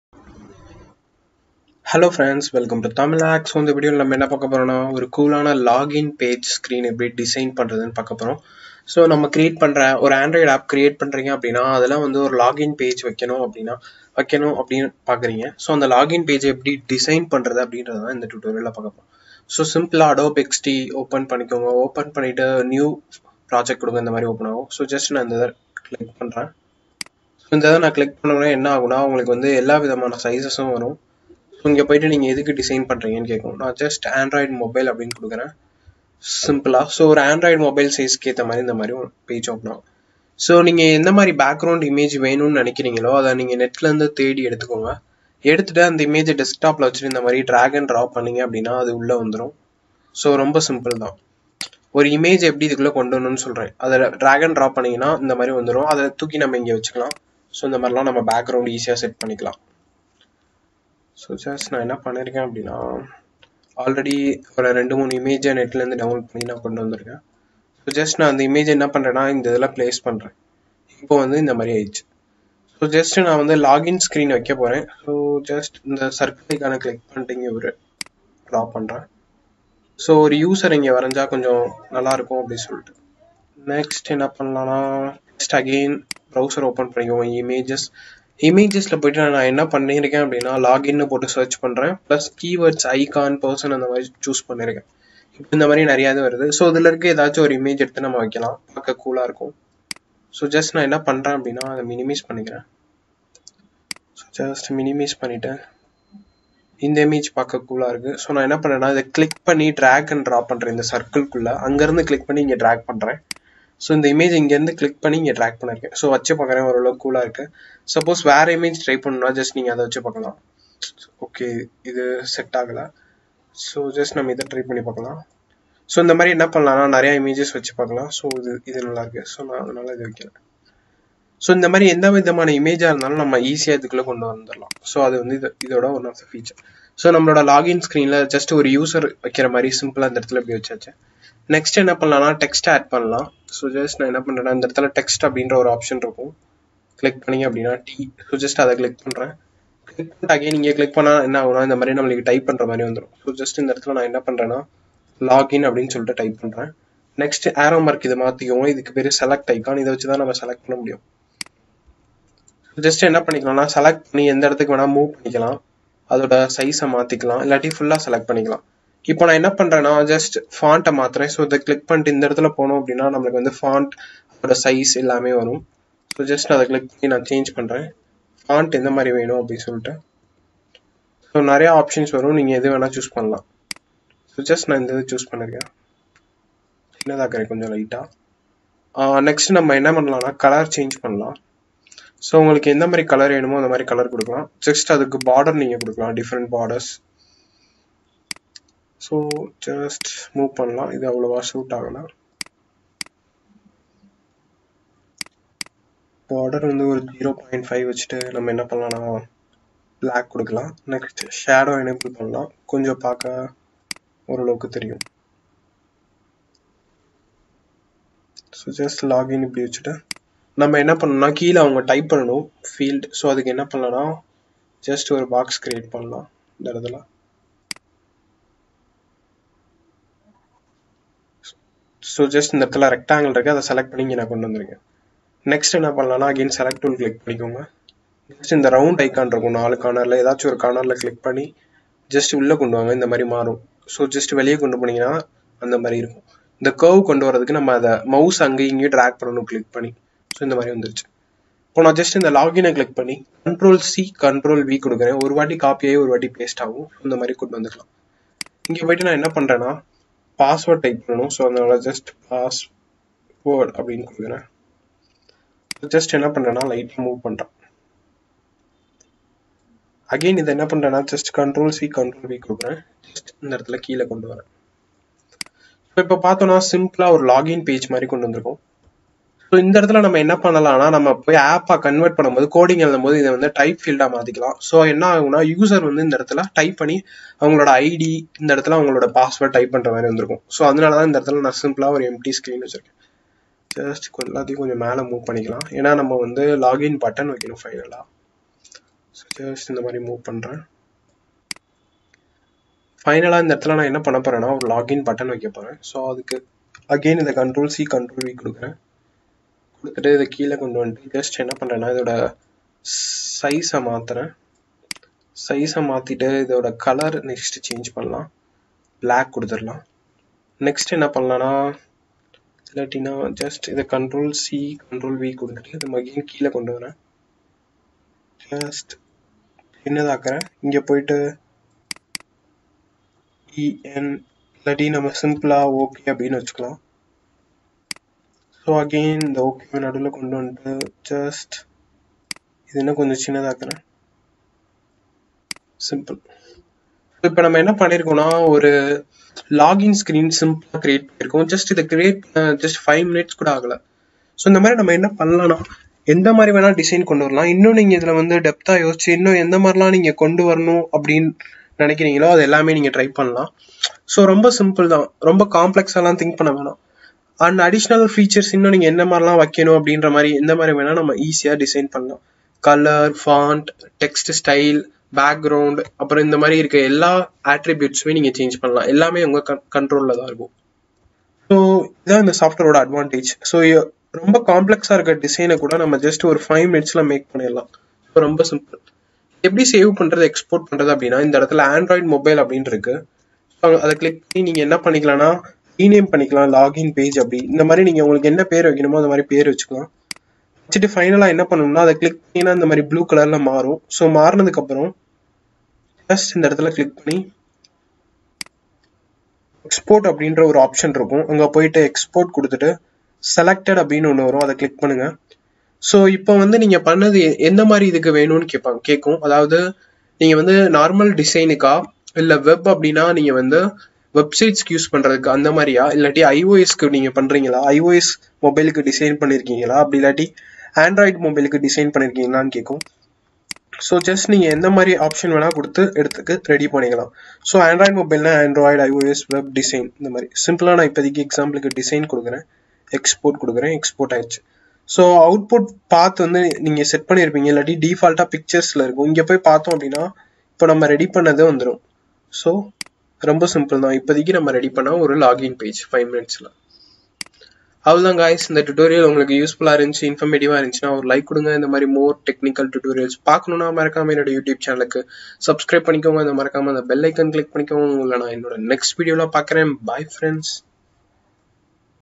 How to design a Login page in 5 mins | adobe xd tutorial | tamil hacks
Hi guys welcome to Tamil hacks 👨‍💻
Learn something new everyday ❤

and more topics available ,check out Tamil hacks

Give this video a Thumbs Up 👍